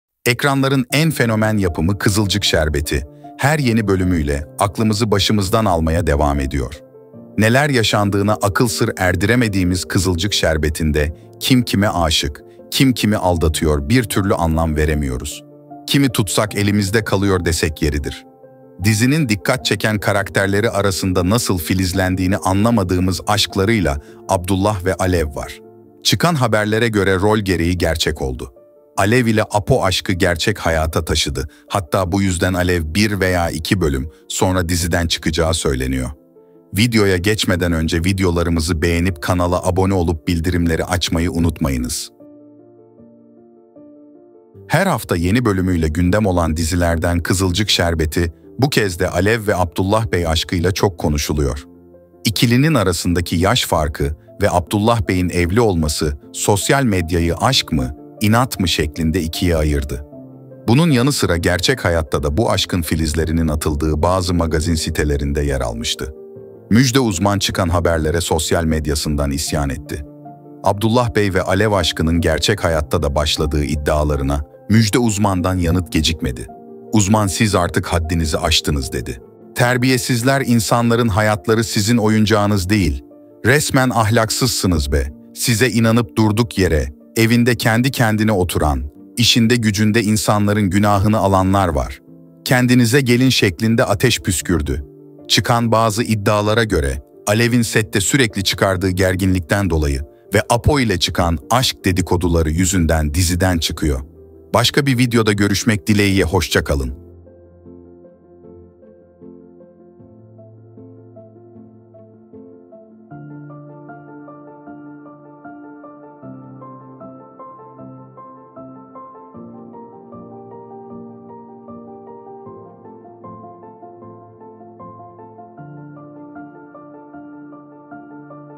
Kızılcık Şerbeti Alev Abdullah Yüzünden Diziden Ayrıldı 😭 | 52.Bölüm
Her hafta yeni bölümüyle gündem olan dizilerden Kızılcık Şerbeti bu kez de Alev ve Abdullah Bey aşkıyla çok konuşuluyor. İkilinin arasındaki yaş farkı ve Abdullah Bey'in evli olması sosyal medyayı Aşk mı, İnat mı? şeklinde ikiye ayırdı. Bunun yanı sıra gerçek hayatta da bu aşkın filizlerinin atıldığı bazı magazin sitelerinde yer almıştı. kızılcık şerbeti alev abdullah diziden ayrıldı.